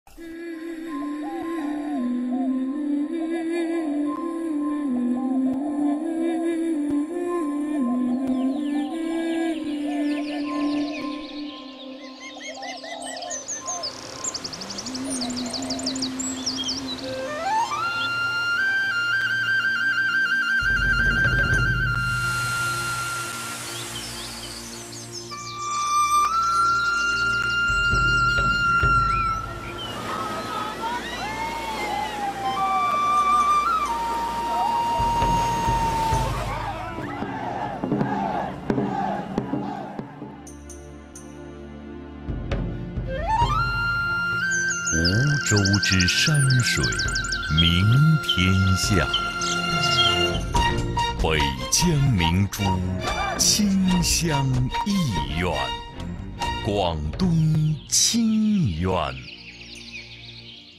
Great America "Qingyuan"
Qingyuan unique scenery of mountains and canyons, rivers, lakes, forests, caves and hot springs strange landscape. Attractions many, Feixia scenic spots, fresh hot mineral spring, treasure Crystal Palace, England Xifeng forest, even the state underground river, Huang River Three Gorges, three rows yaozhai large Asahi mountain waterfalls, emerging with "Tang Buddhist" as the subject of the build the Shaolin Temple and Royal King Street Phoenix TV and so on. Whether you are rafting, adventure, or a sightseeing tour, or hot springs, dragon boat racing, eating food, Qingyuan not let you down.